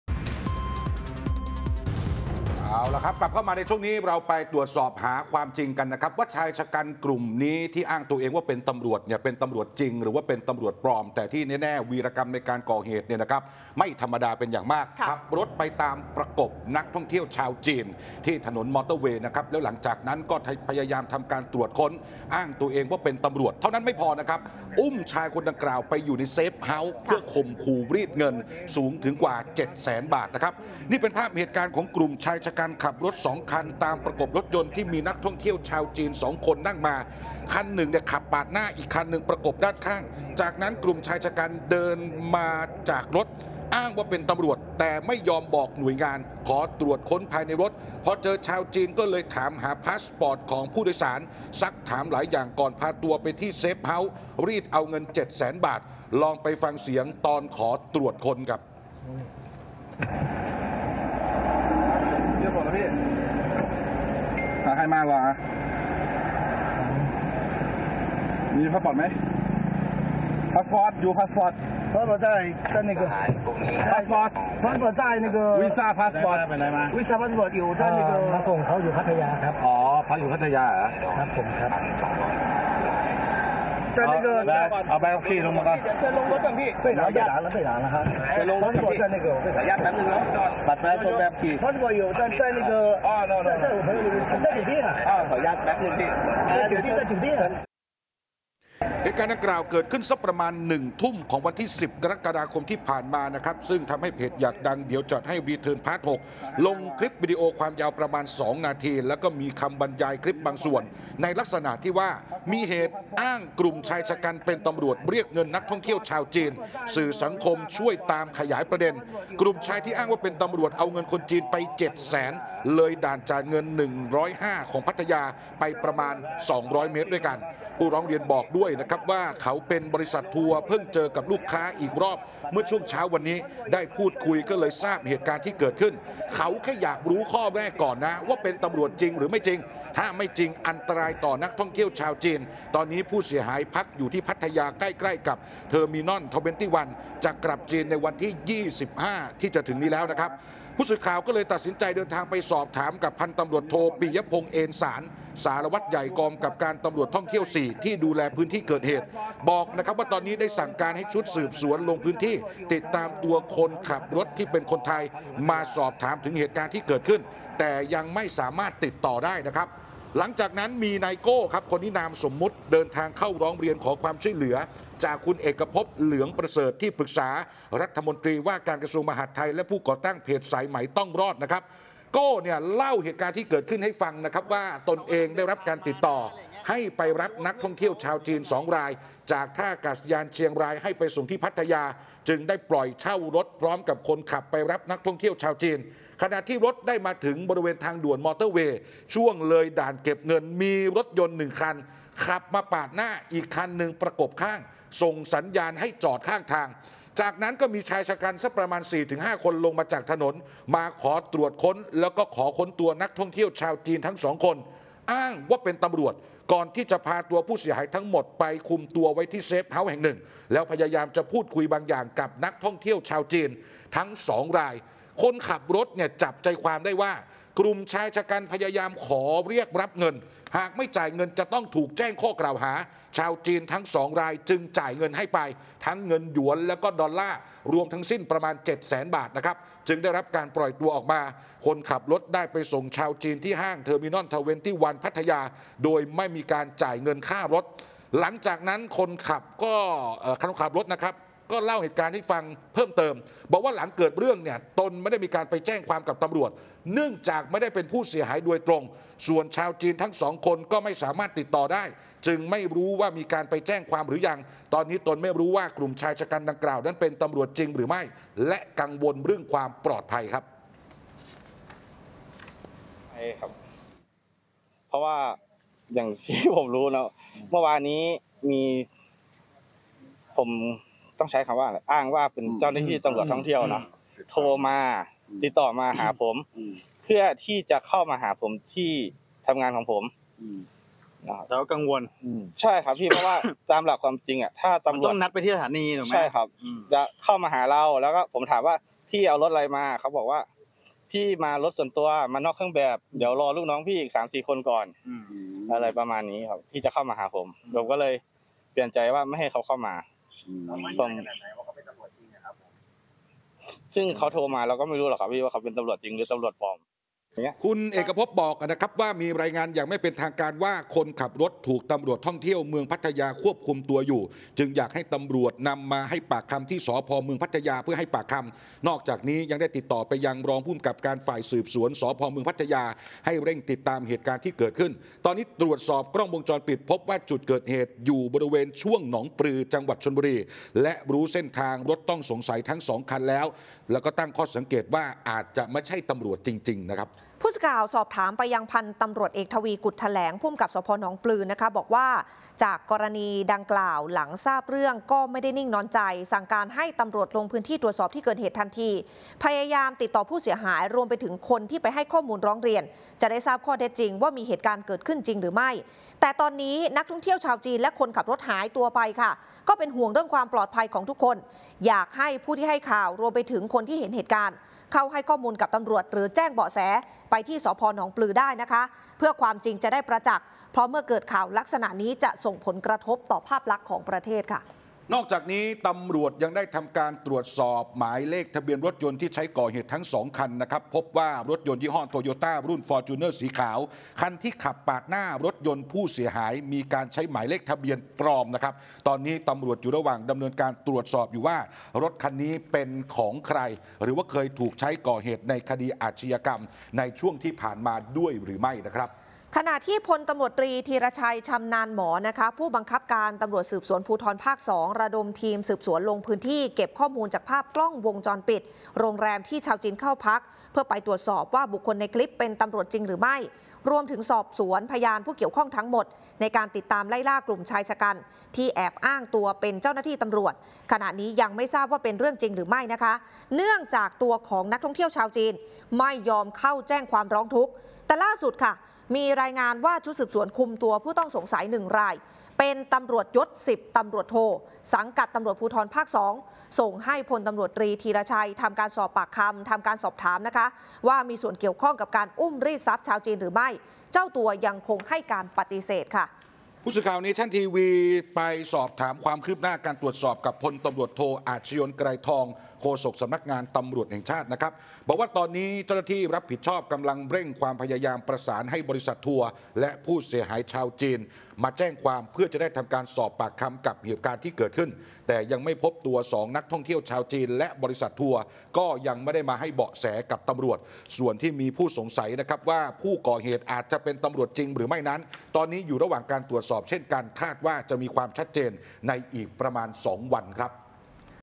อ้างเป็น ตร.ขับรถประกบ อุ้มชาวจีนรีดเงิน 7 แสน | เนชั่นทันข่าวค่ำ | NationTV22
เกิดเหตุอุกอาจกลางถนนมอเตอร์เวย์ กลุ่มชายฉกรรจ์ขับรถยนต์ตามประกบรถนักท่องเที่ยวชาวจีน บังคับให้จอดข้างทาง อ้างเป็นตำรวจขอตรวจพาสปอร์ต แล้วพาตัวไปที่เซฟเฮ้าส์ รีดเอาเงินไป 7 แสนบาท ก่อนปล่อยตัวออกมา